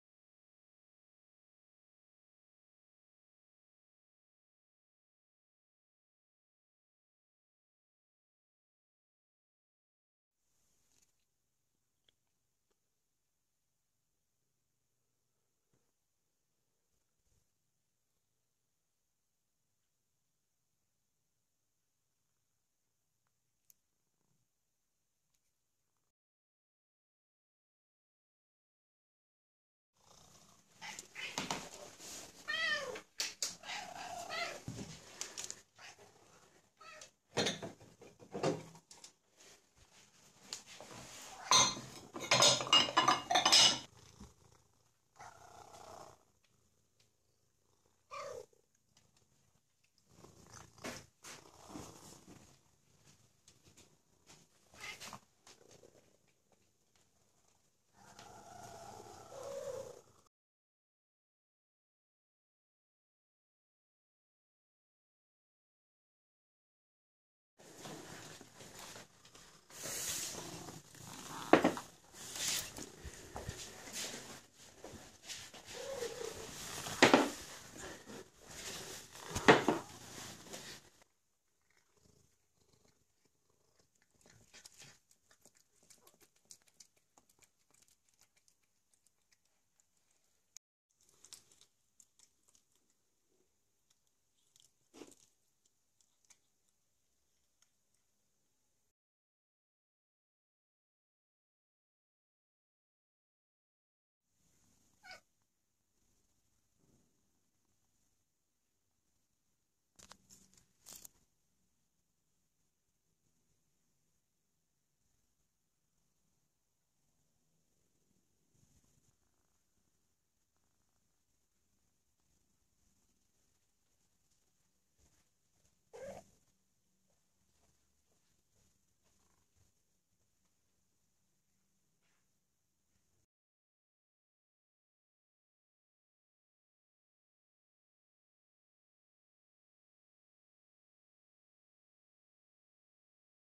Having cats is awesome...
Having cats is awesome, because you will probably never oversleep again.